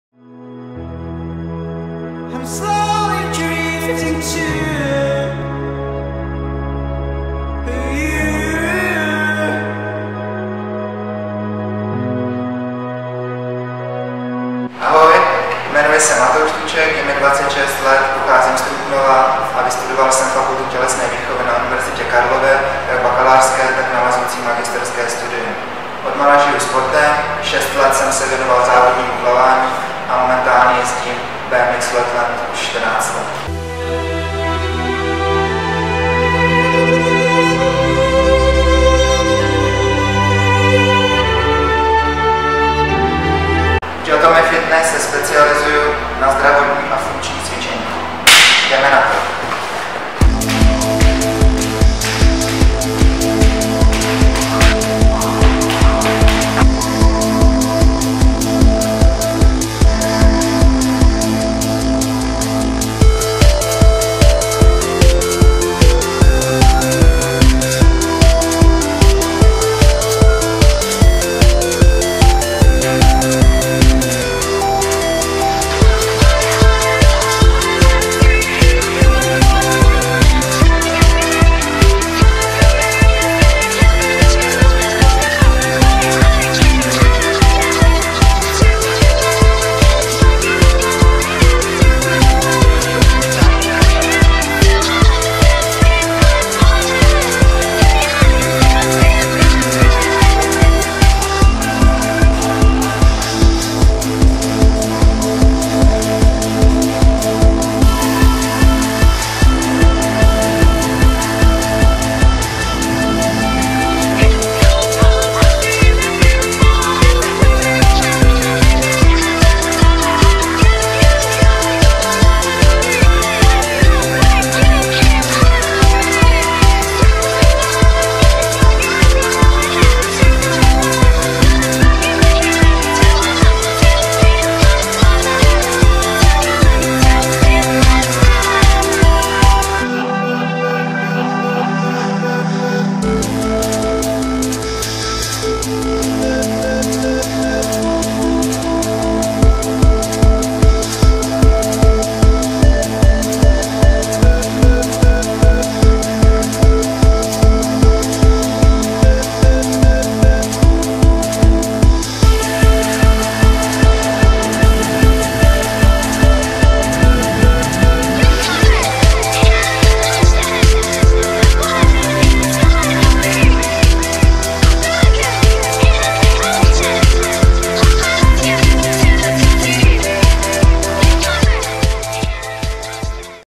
Matouš Tuček - Jatomi Fitness Palladium
Osobní trenér Matouš Tuček
Specializace - zdravotní a funkční cvičení
Jatomi Fitness - Palladium Praha

Video by Michal Kupec | @michalkupec